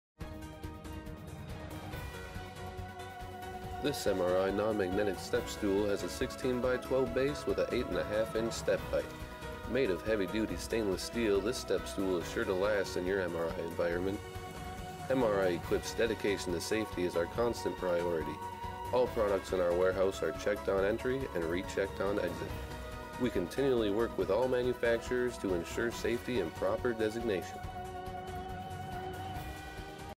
MRI Safety Step Stool
This is the MRI Non-Magnetic Step Stool. Perfect for your MRI environment. It is stainless steel so it resists the normal wear and tear of daily activity. You can finally have a stool in you MRI room without worrying about the risk.